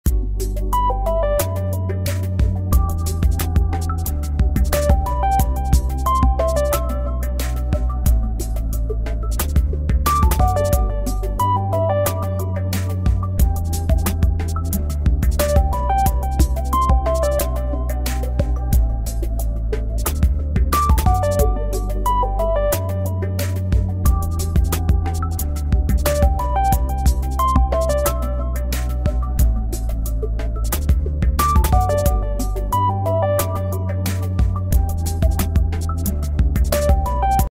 Pokemon EB5AA Monsteroctober Creatures - Bubbleluff
5th of October
Bubbleluff is fusion of Bubble Crab and Chick.
It ability to put the ingredients into the pot to make Special Cards and Powers inside the bubbles.